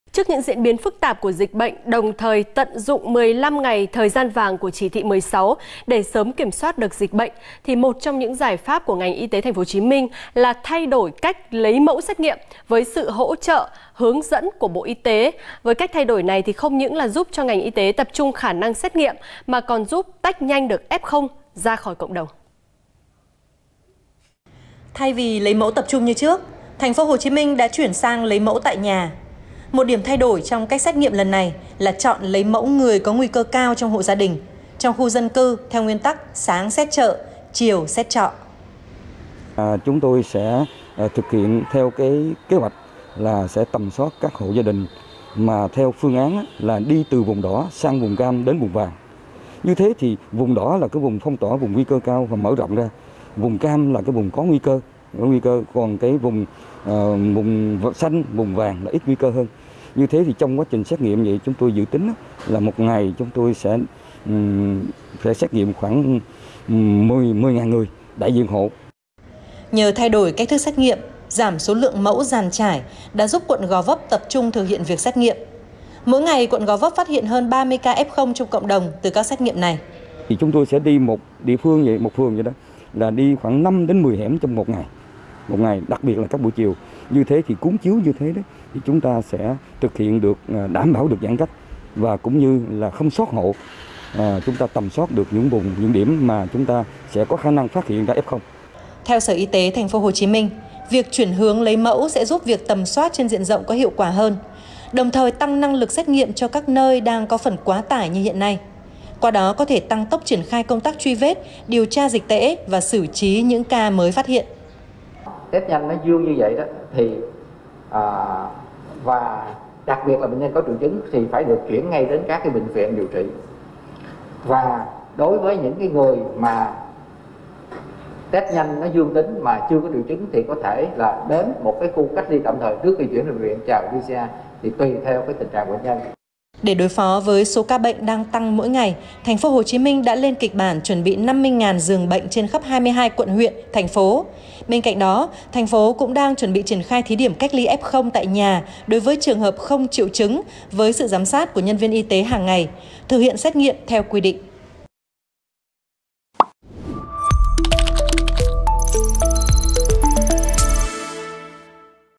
TP Hồ Chí Minh: Thay đổi cách xét nghiệm nhằm phân tách F0 | VTV24
Để nhanh chóng kiểm soát tình hình dịch bệnh trên địa bàn, đồng thời tận dụng 15 ngày thời gian vàng của Chỉ thị 16, một trong những giải pháp của ngành y tế TP Hồ Chí Minh là thay đổi cách lấy mẫu xét nghiệm theo hướng dẫn của Bộ Y tế.